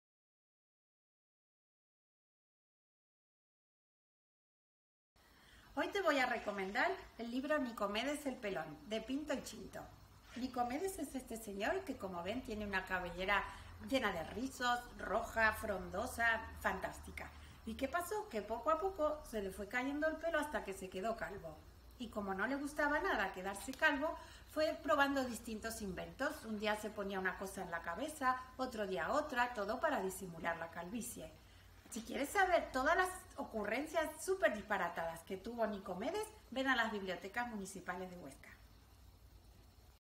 Cada día un cuento: "Nicomedes el pelón" de Pinto&Chinto
Nuestras bibliotecarias te recomiendan cuentos que podrás encontrar en las bibliotecas del Ayuntamiento de Huesca.